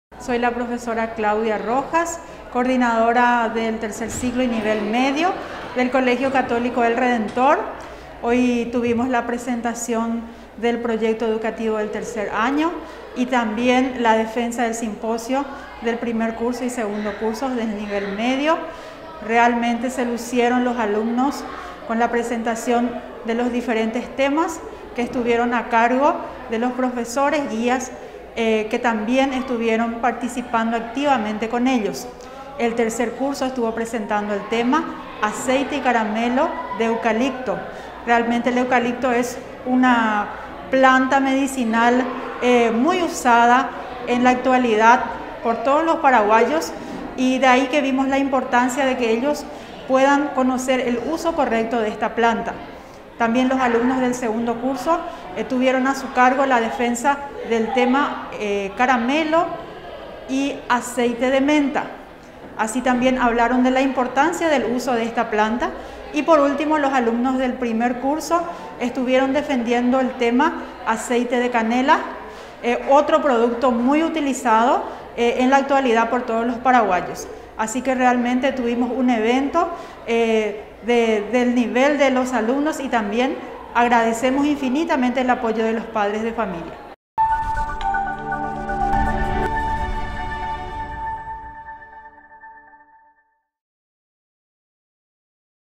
PRESENTACION DE PROYECTO DEL 3ER. AÑOCOL. CATOLICO EL REDENTOR
DEFENSA DE SIMPOSIO DEL 1RO. Y 2DO. CURSOS
COL. CATOLICO EL REDENTOR